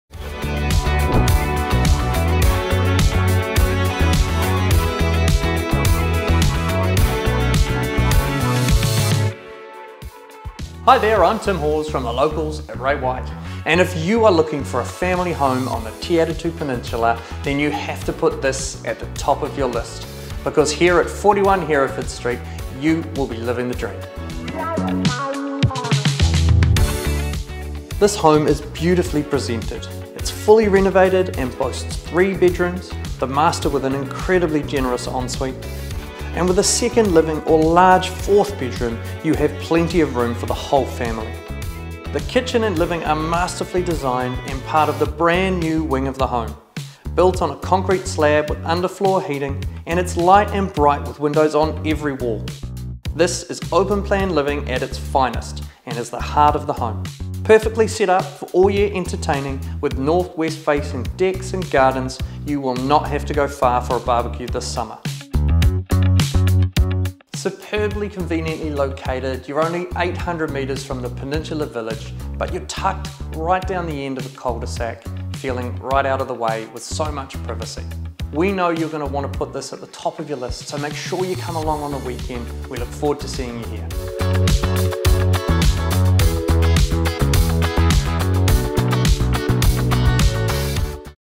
41 Hereford Street, Te Atatu Peninsula | The Locals at Ray White Kingsland
A Flawless Find For Families

Get ready to feel the love! This stunning home provides the ultimate in first class family living and entertaining. Designed with flair and presented beautifully from top to toe, the spacious accommodation will impress as will the careful attention to detail on display both inside and out. The single level floor plan is perfectly set up for your year round entertaining pleasure with sun soaked north west facing decks and secure landscaped and level 726 sqm grounds for young and old to enjoy.

The fabulous open plan kitchen and dining zone, complete with underfloor heating beneath gleaming concrete floors, is the ideal spot for informal family get-togethers or larger gatherings of the whole clan. The sleek and sophisticated extension and renovations have created a super stylish vibe and we know you're going to be so proud to call this remarkable residence home. The three bedrooms are all well sized, the master enjoying a superb ensuite, plus a gorgeous family bathroom. A second formal living area is so versatile - use it as your media room, a welcome and cosy retreat in the cooler months or as a fourth bedroom if that suits your needs.

Positioned in a peaceful cul de sac just 800 metres from the heart of the village, there's a smorgasbord of amenities to make the most of here. Popular local cafes and restaurants, supermarkets, public transport, lovely parks - and of course the beach - are all so close, along with easy access to motorway links. Your property prayers have really been answered this week - see us at the open homes this weekend and get ready to start living the Peninsula dream!